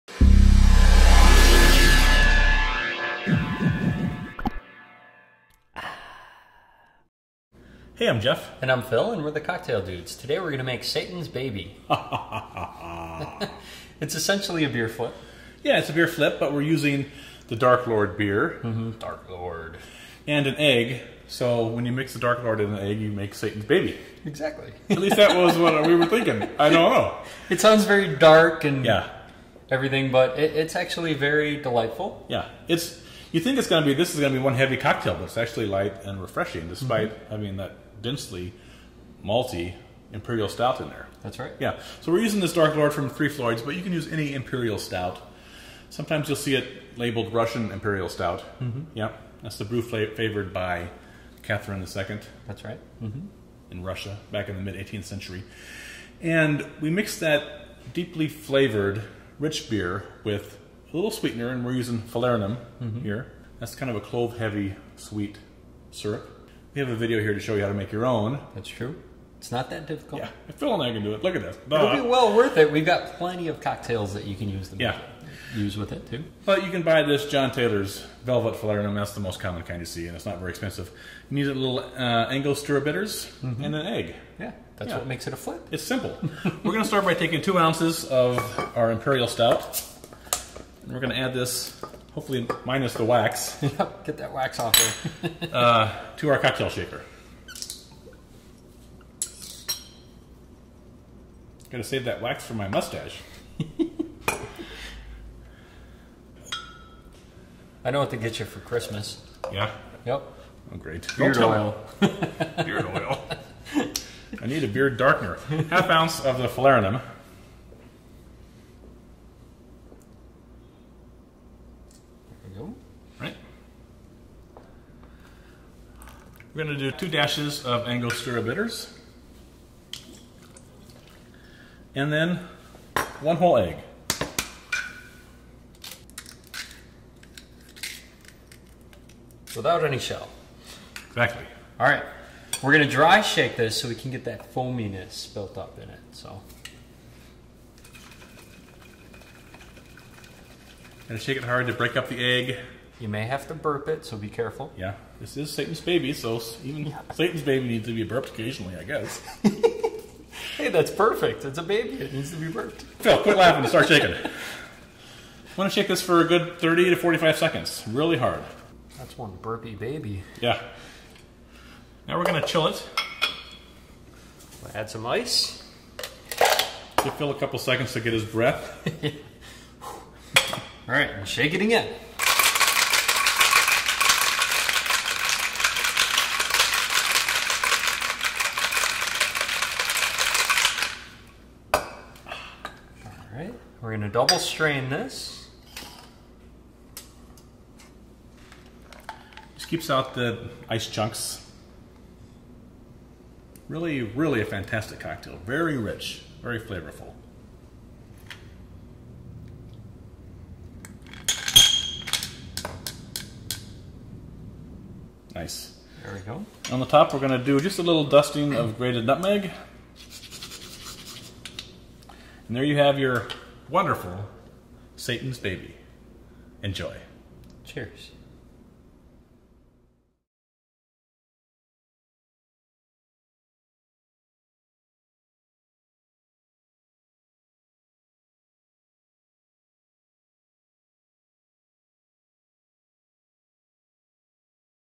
Satan's Baby from the Cocktail Dudes
This cocktail is essentially a Beer Flip. We take Three Floyd’s DARK LORD beer and shake that with an egg to create Satan’s Baby. Though it sounds dark and woeful, it is, in truth, a rather light-feeling, delightfully rich, velvety beer cocktail with wonderfully fragrant and spicy notes. Three Floyd’s DARK LORD is an Imperial Stout – oftentimes called a Russian Imperial Stout, being the rich, malty, brew favored by Catherine II’s Russian court. Adding Falernum and egg to an Imperial Stout really accentuates and draws out the vanilla, caramel, spice, and other complex notes of the dense beer. It makes for a fun, creamy, and very enjoyable beer cocktail. And although you should never shake a baby, when you shake this one, be certain to burp it initially as the pressure from shaking the egg will build up in your Cocktail Shaker. You might also like to check out some of the other Falernum cocktails we have made and learn how you can make your own Falernum, as well. Cheers!